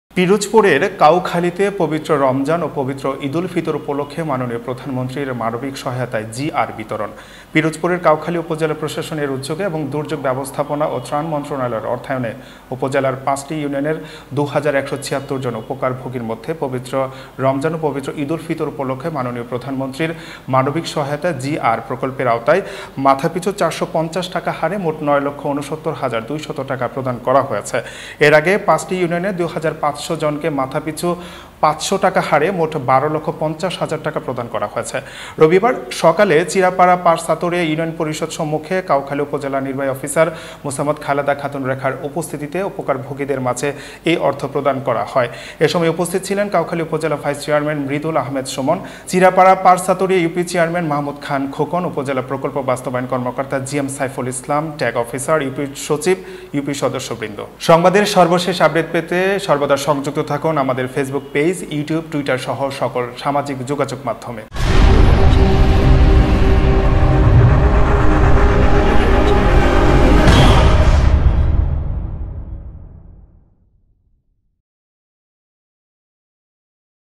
পিরোজপুরের কাউখালীতে পবিত্র রমজান ও পবিত্র ঈদুল ফিতর উপলক্ষে মাননীয় প্রধানমন্ত্রীর মানবিক সহায়তা mp4
পিরোজপুরের কাউখালীতে পবিত্র রমজান ও পবিত্র ঈদুল ফিতর উপলক্ষে মাননীয় প্রধানমন্ত্রীর মানবিক সহায়তা জি.আর (ক্যাশ) বিতরণ।